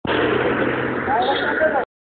A man selling fruits
A man selling different fruits like apple,banana,grapes,pampas,etc.